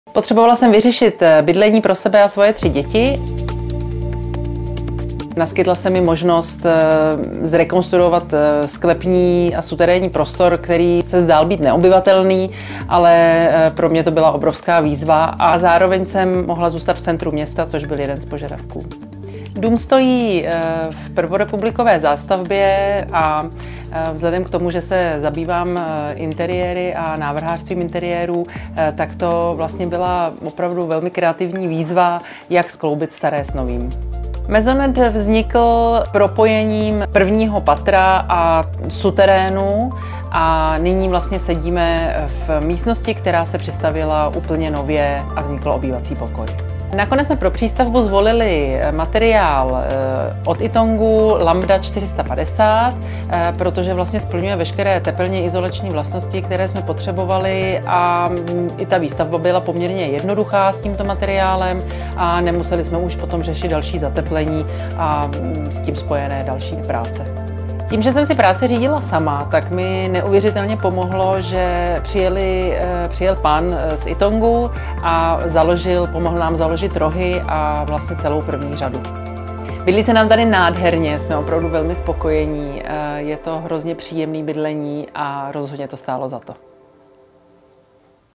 Rekonstrukce domu v prvorepublikové zástavbě | Ytong
Jak dopadla přístavba pod taktovkou paní Magdaleny? Podívejte se sami, jak se jí Lambda YQ osvědčila. Nám se nové bydlení moc líbí a má styl. Více na